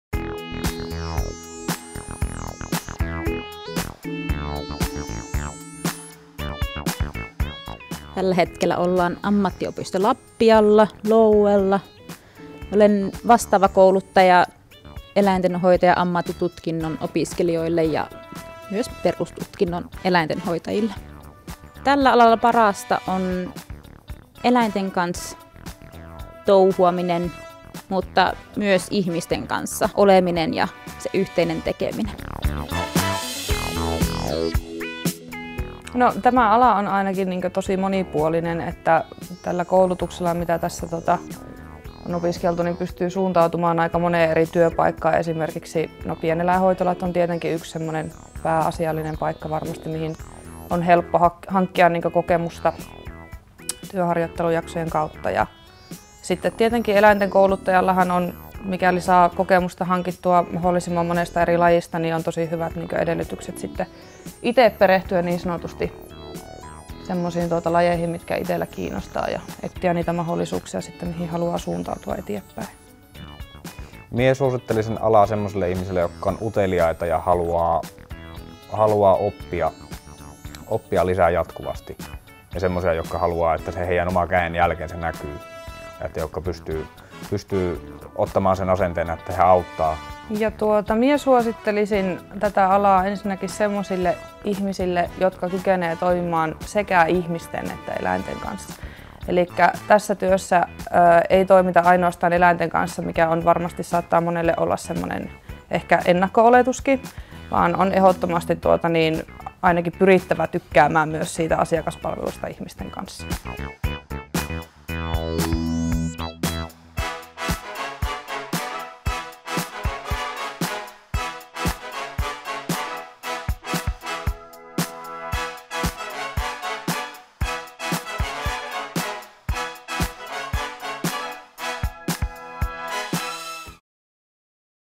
Eläintenhoitaja
Louella voit opiskella maatalousalan perustutkinnon, eläintenhoitajan osaamisalan sekä eläintenhoitajan ammattitutkinnon.